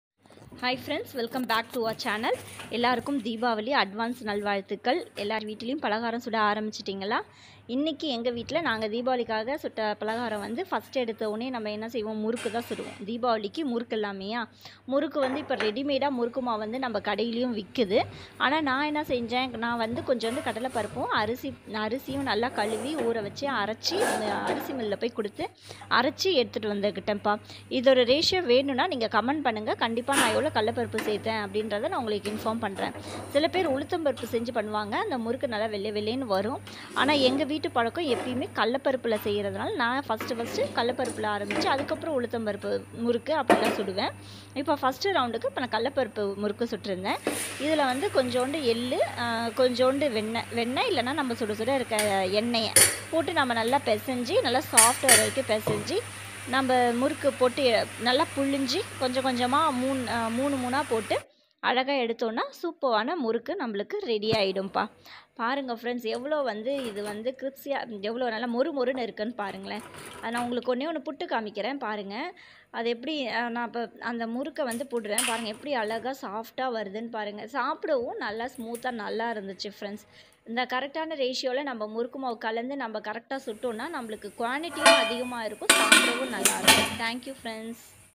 Diwali celebration starts #14
Diwali celebration starts here i had captured my muruku video

Mahe's lifestyle share my ideas in cooking,gardeing,digital etc...